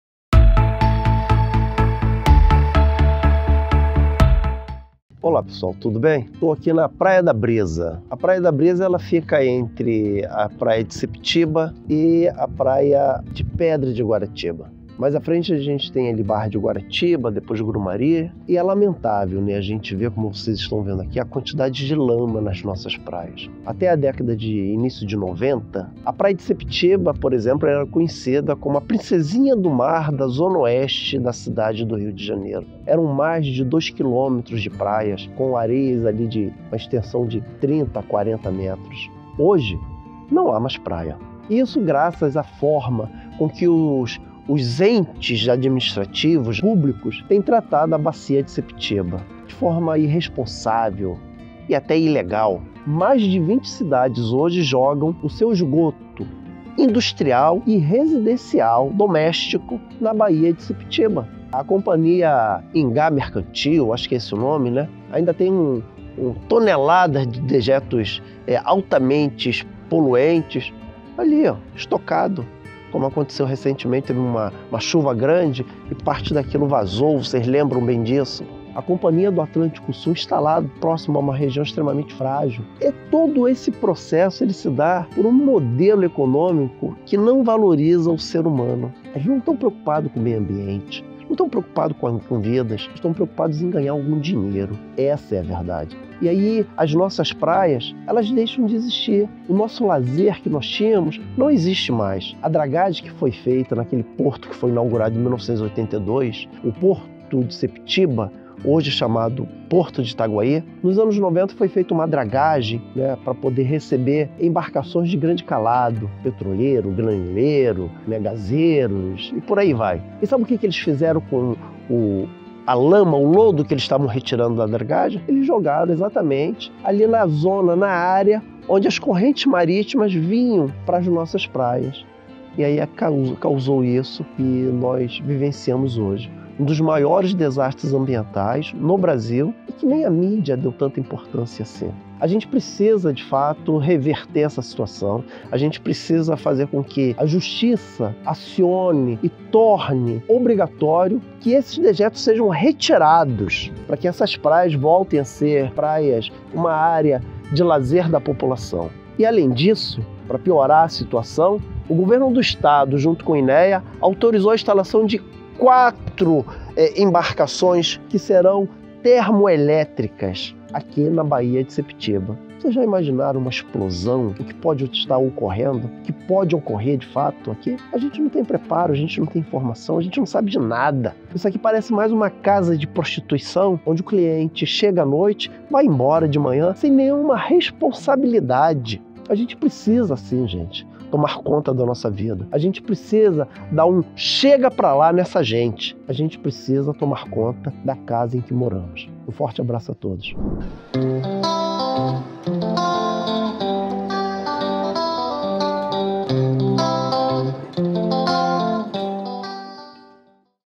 A BAÍA DE SEPETIBA PEDE SOCORRO! DEGRADAÇÃO AMBIENTAL
ALEXANDRE PANTALEÃO FALANDO SOBRE A DEGRADAÇÃO DA BAÍA DE SEPETIBA E SOBRE O DESCASO DO PODER PÚBLICO COM A "PRINCESINHA" DA ZONA OESTE.